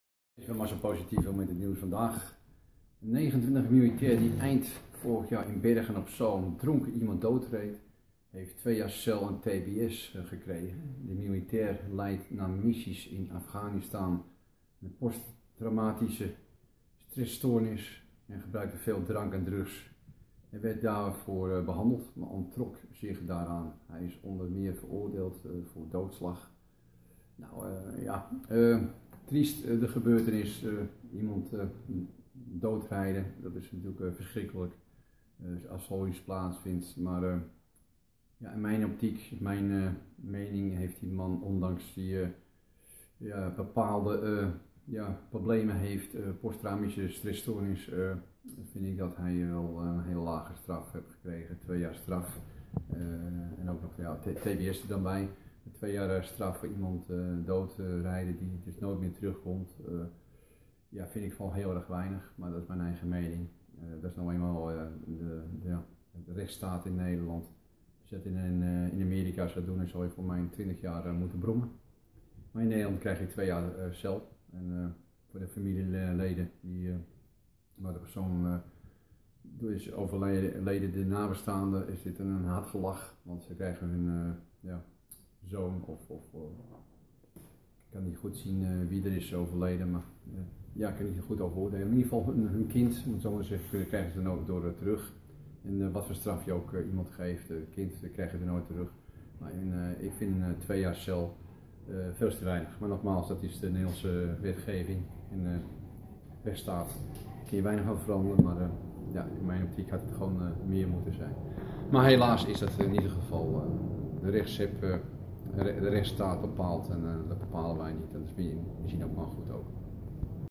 Militair krijgt 2 jaar cel naar doodrijden van iemand.
2 jaar cel en TBS na doodrijden persoon